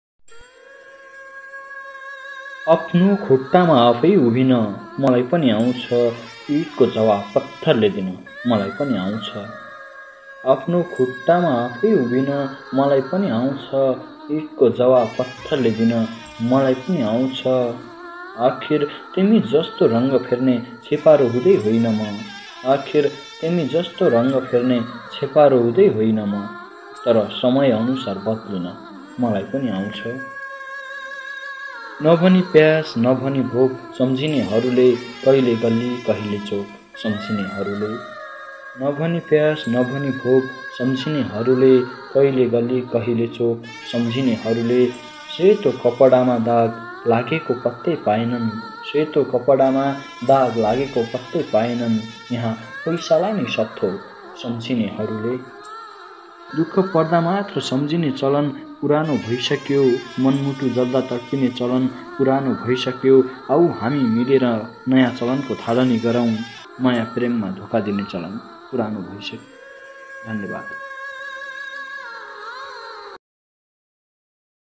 सुरेश जाग्री ।।सेतो कपडामा दाग लागेको पत्तै पाएनन्। यहाँ पैसालाई नै सबथोक सम्झिनेहरूले! वाह मुक्तक
नभनी  प्यास नभनी भोक सम्झिनेहरूले
कहिले गल्लि कहिले चोक सम्झिनेहरूले
सेतो कपडामा दाग लागेको पत्तै पाएनन्
यहाँ  पैसालाई नै सबथोक सम्झिनेहरूले!